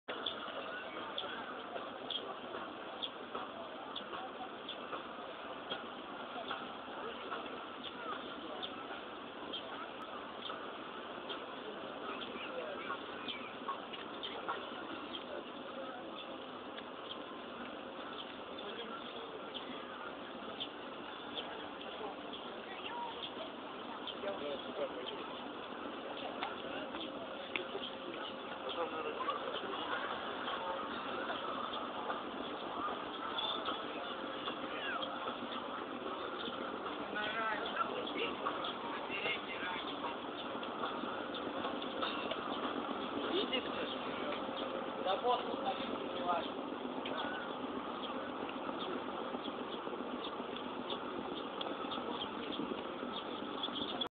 Прогулочный катер на Амуре
Прогулочный катер по река Амур в Хабаровске.